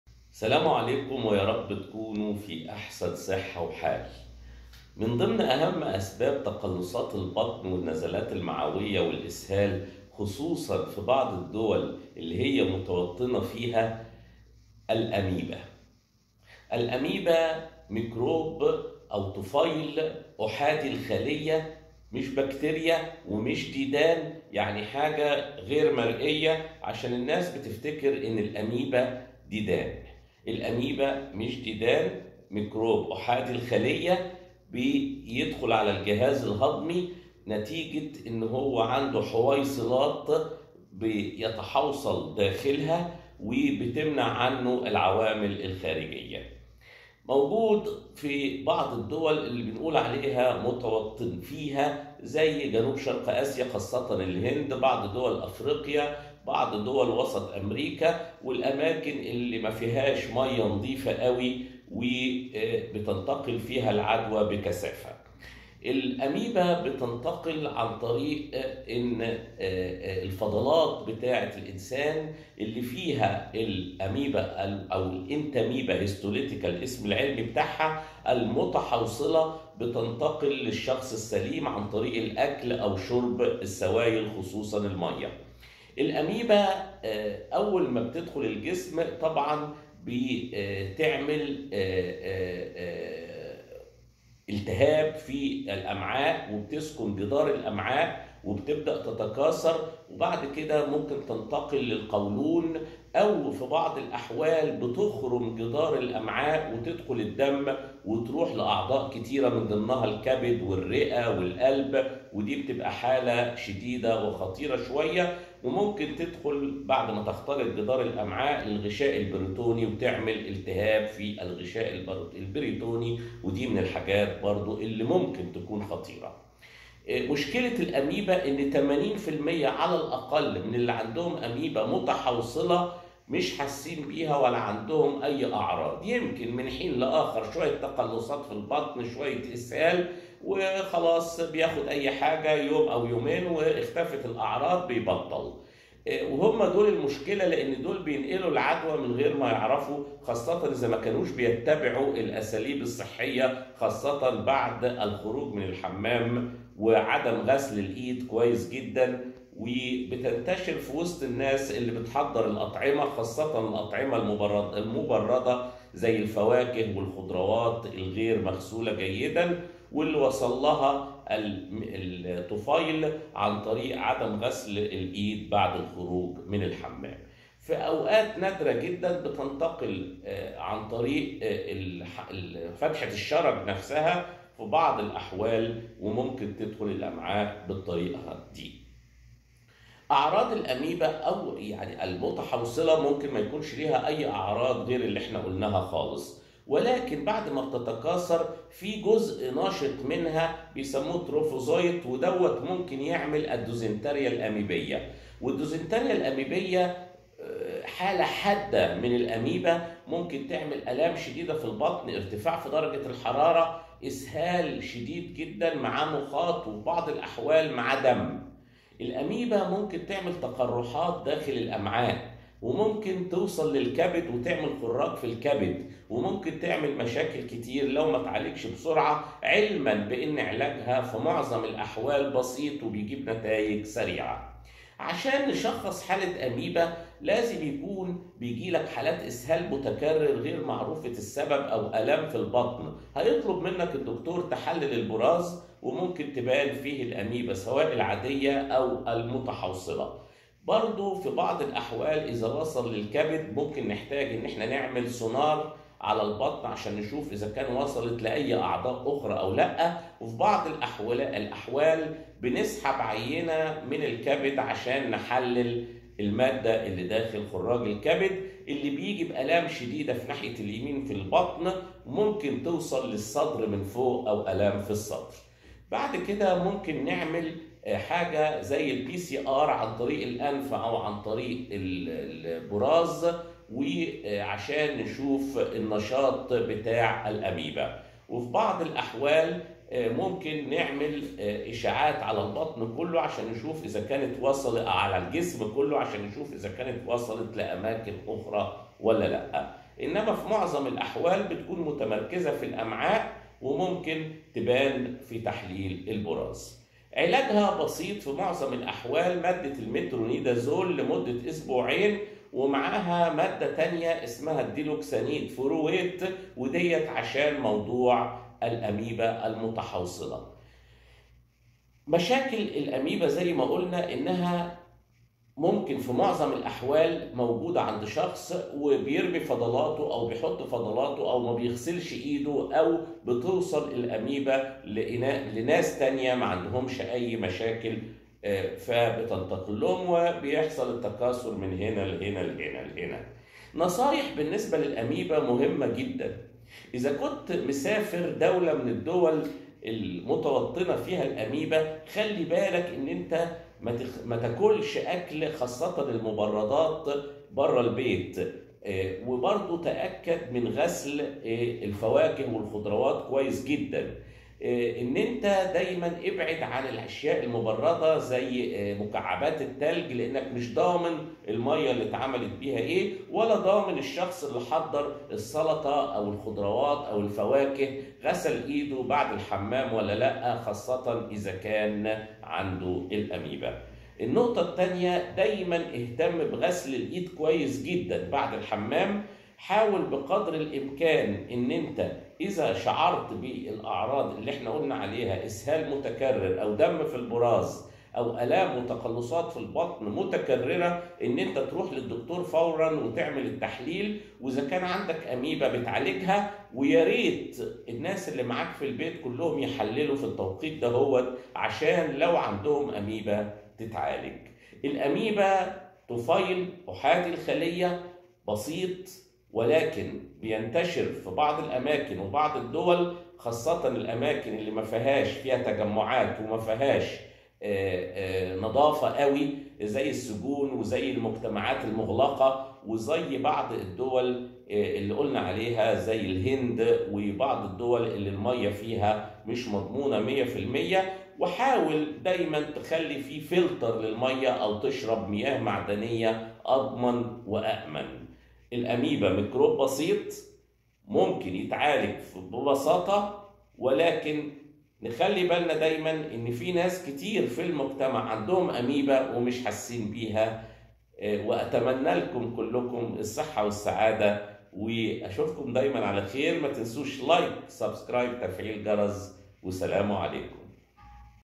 الاميبا و الدوسنتاريا الاميبية. Amoebiasis.
شرح للاميبا و علاجها.
amoeba and management.